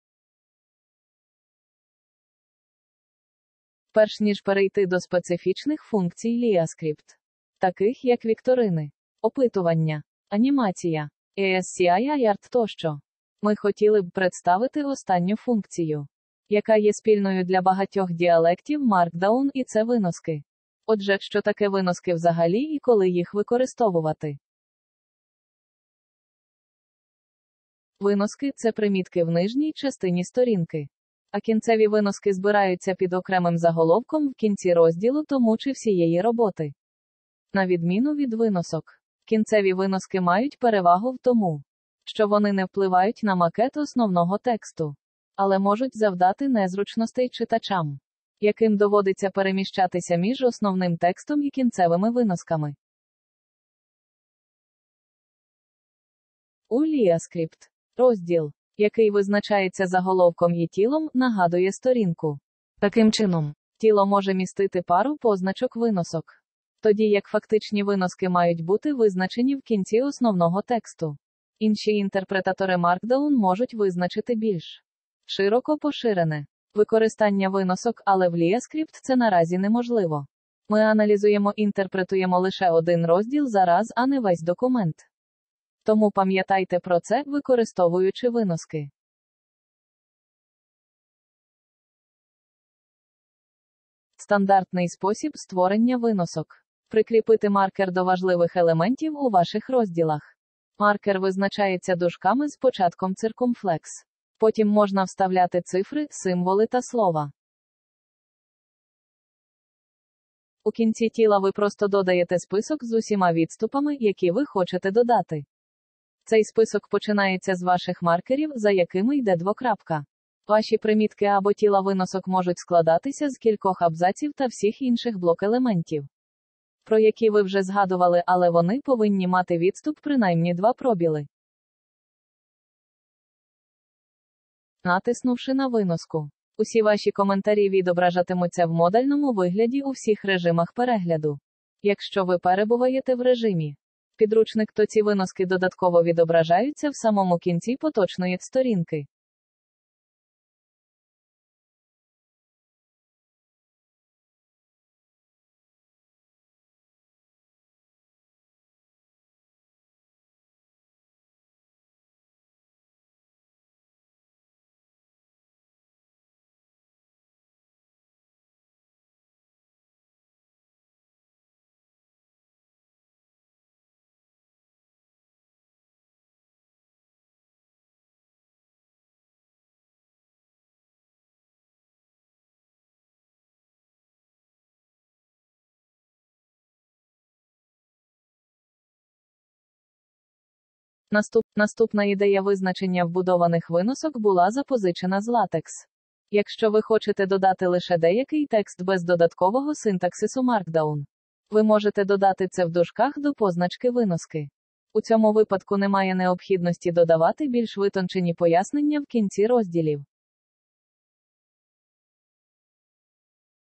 LiaScript0.10: Footnotes (ukrainian) #6
A complete overview on how to define footnotes and when to use them in LiaScript/Markdown.